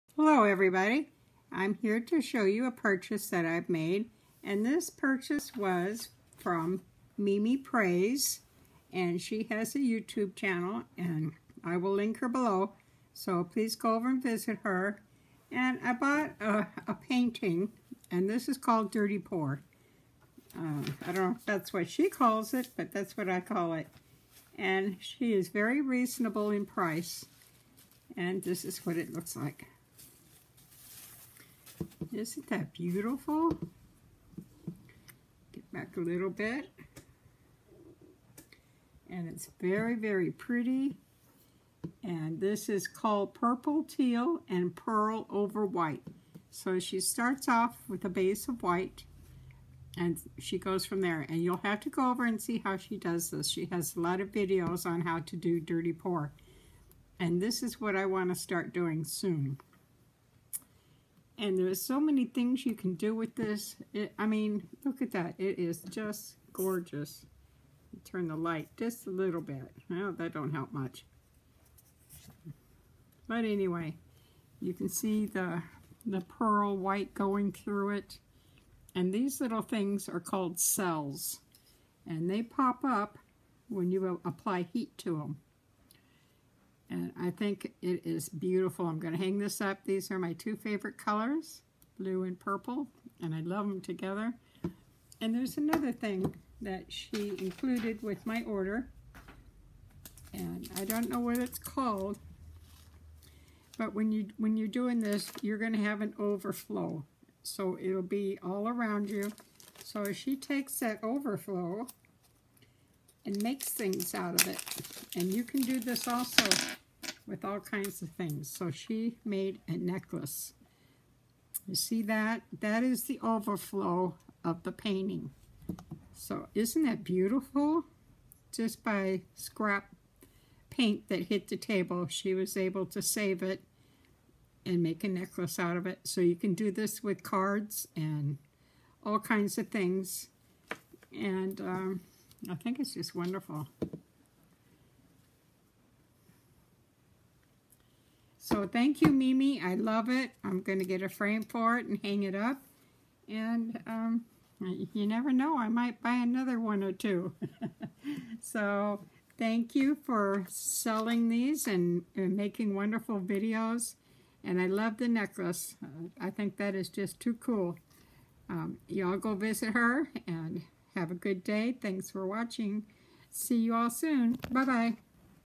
A Special Purchase From Mimi Prays ~ Dirty Pour Painting!
A Special Purchase From Mimi Prays ~ Dirty Pour Painting! Thank you Mimi, this is really a beautiful painting.  Everyone please visit Mimi at this link:
Would love for you to join in. Comments are always welcome!